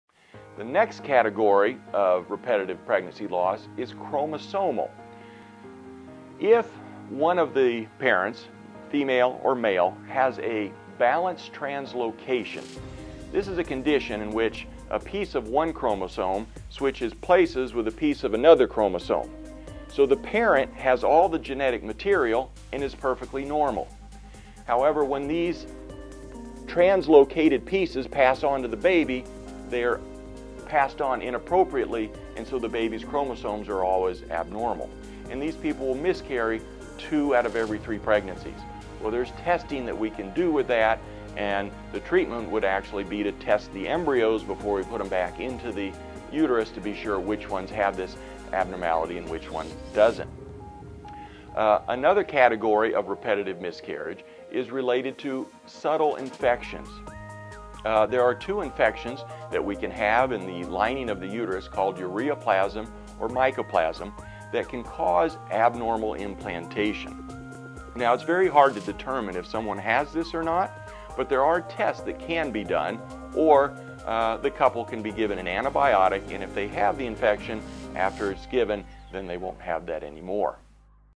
Causes of Recurrent Miscarriage: Chromosomal & Uterine Infections
What causes recurrent miscarriage? Dr James Douglas discusses the diagnosis and causes of multiple miscarriage